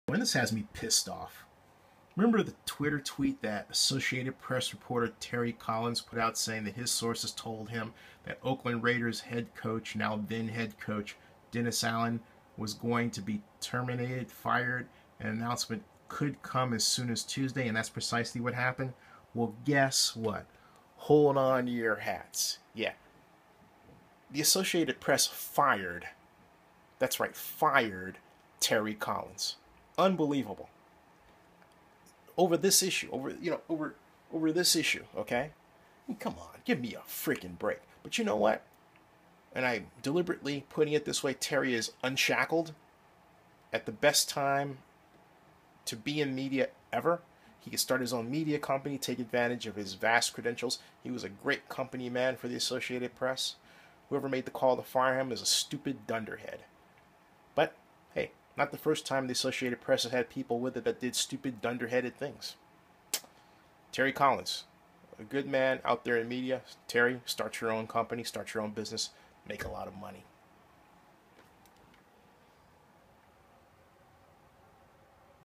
Terry Collins Associated Press Reporter Fired For Dennis Allen Tweet
Zennie62 Terry Collins Associated Press Reporter Fired For Dennis Allen Tweet

Terry Collins, the Associated Press Reporter who tweeted that then-Oakland Raiders Head Coach Dennis Allen was to be fired on Tuesday, was terminated by the AP.

That is right. Terry Collins was fired for the tweet reporting that Dennis Allen was to be fired by the Oakland Raiders, even though he was correct.

Now another very talented black journalist is available for hire or to start his own media company.  The AP person who made that call made a stupid move, but that is not a first for a person working for the Associated Press.

- vlog produced by Zennie Abraham Zennie62 Blog - youtube video - Oakland Raiders highlights and commentary